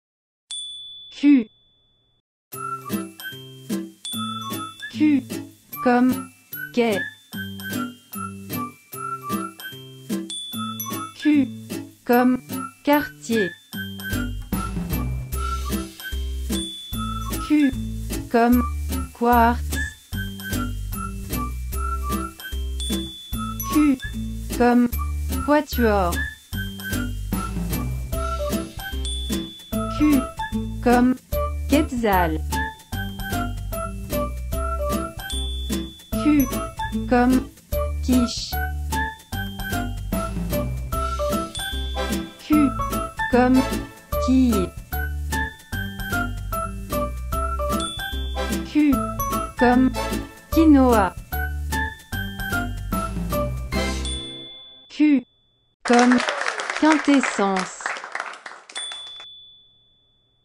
Alphabet français - la lettre Q
Apprenons les lettres de l'alphabet. Pour les petits écoliers. La lettre Q.